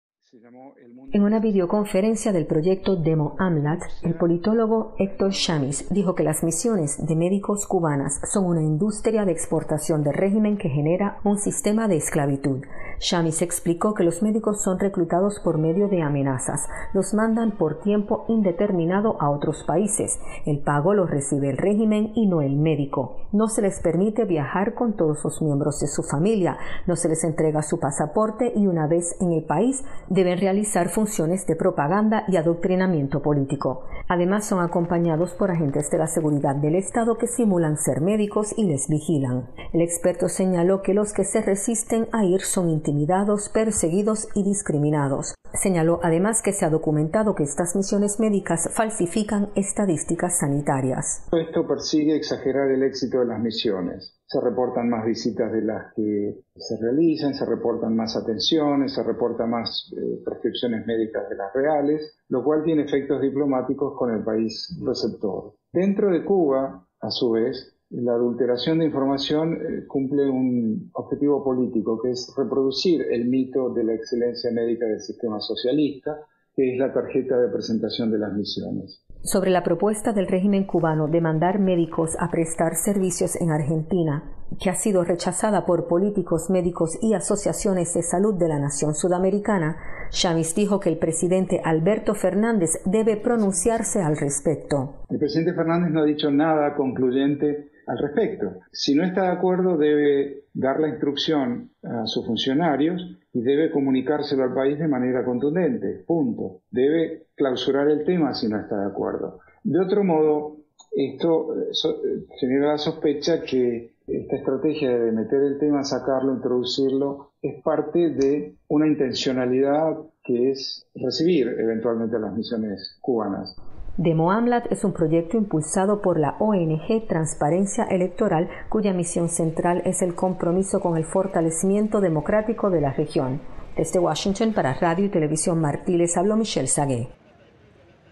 Misiones médicas son calificadas como "trabajo forzoso" por la ONU
El régimen cubano ha usado la crisis del coronavirus para impulsar las misiones médicas, que fueron calificadas como "trabajo forzoso" por Naciones Unidas, según afirmó en una vídeo-conferencia, Hector Schamis, profesor adjunto de la Universidad de Georgetown, en Washington DC